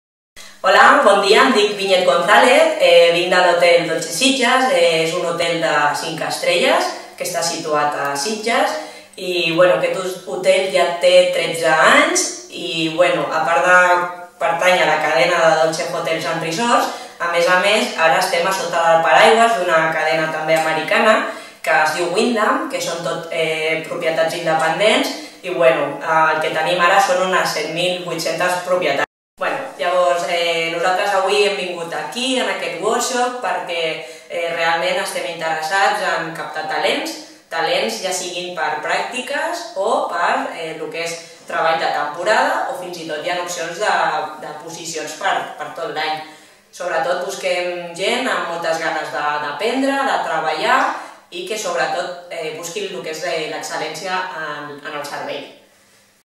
Vinyet González de Hotel Dolce Sitges. II Jornada FTG Feina i Futur
Vinyet González, tècnica de recursos humans de l'Hotel Dolce Sitges, ens explica quin és el perfil professional requerit per la seva organització en el marc de la II Jornada FTG Feina i Futur.